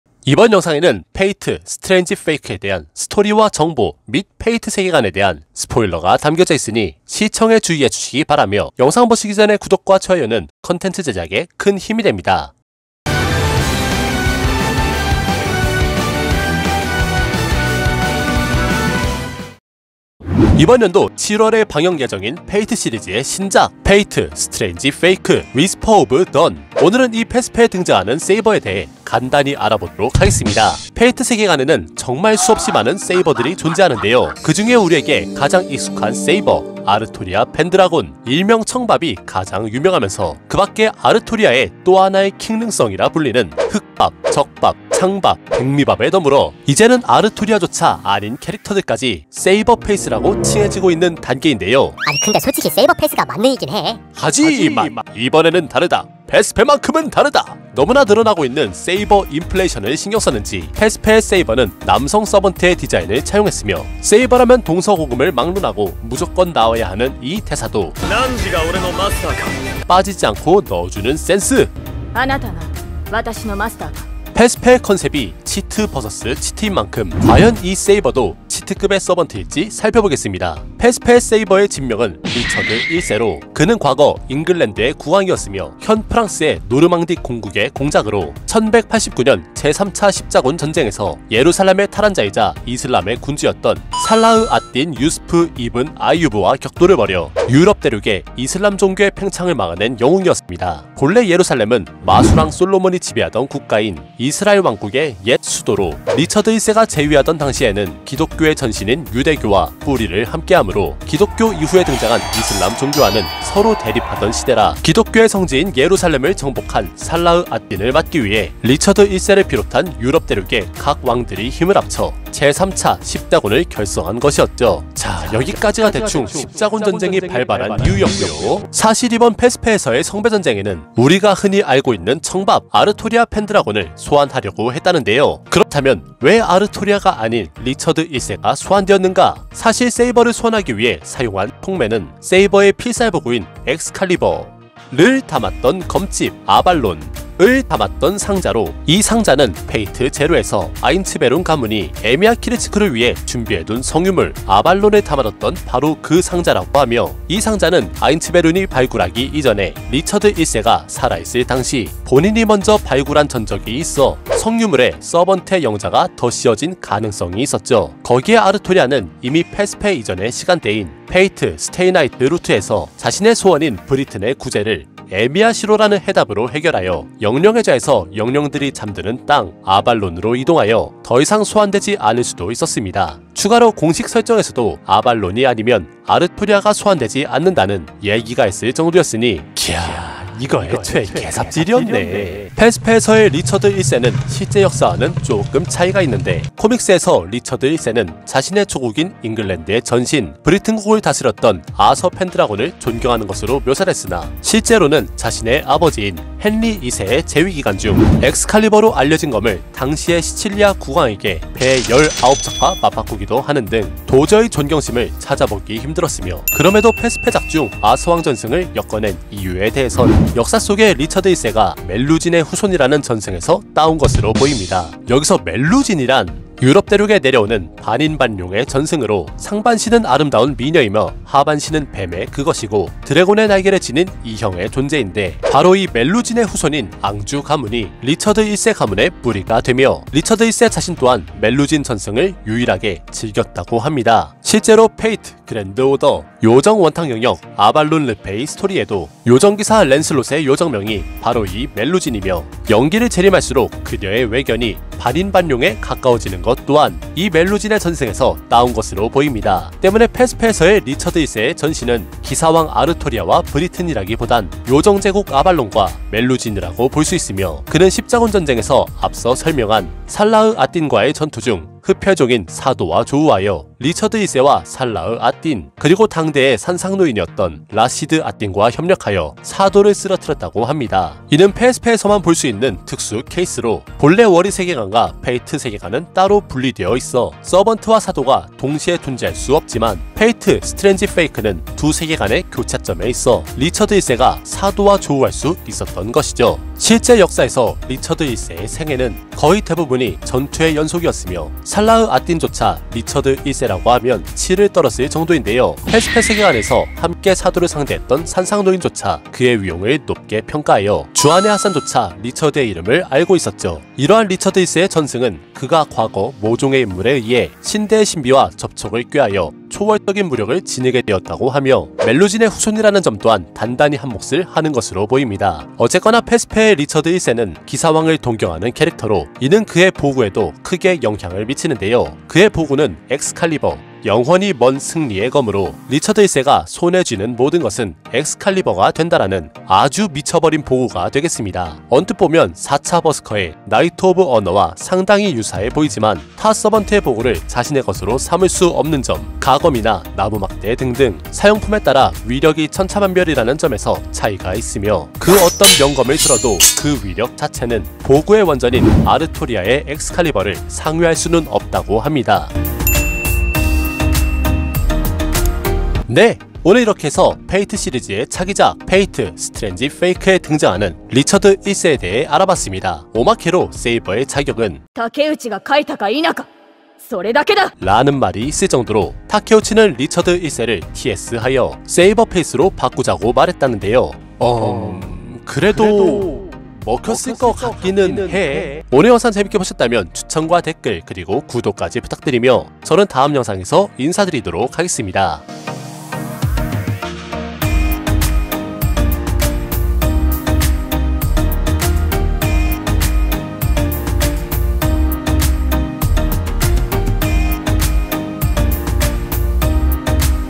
드래곤의 혈통으로 중세를 평정한 X깡패 임금님. 페이트 스트레인지 페이크 리처드 1세 프리뷰!! Fate strange Fake Preview
7월에 방영 예정인 페이트 스트레인지 페이크의 뉴페이스, 세이버!
사자심왕 리처드 1세에 대해 알아봤습니다!